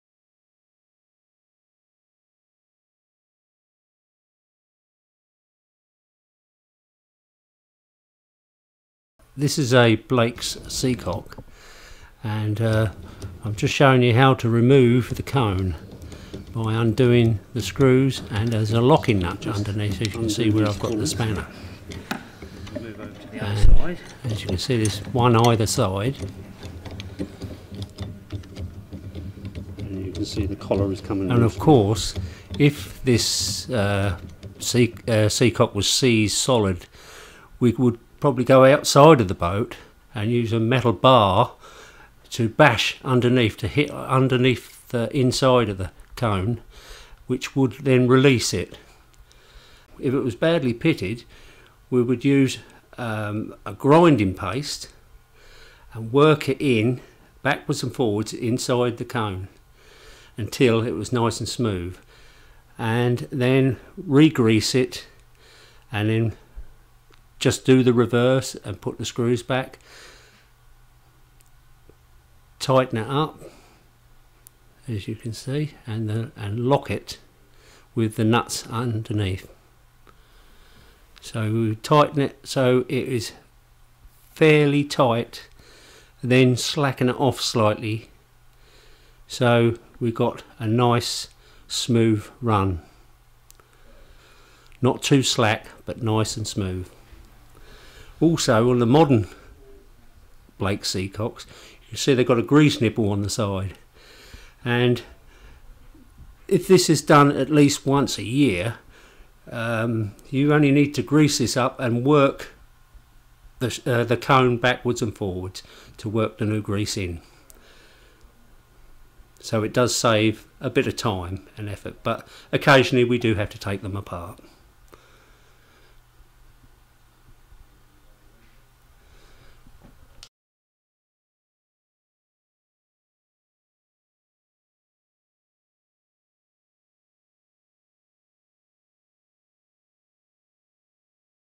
Servicing or reconditioning a seacock How-to
An essential part of keeping your wooden boat in commission – watch this short video to find out how to service a seacock as shown by a member of Suffolk Yacht Harbour's boat building team.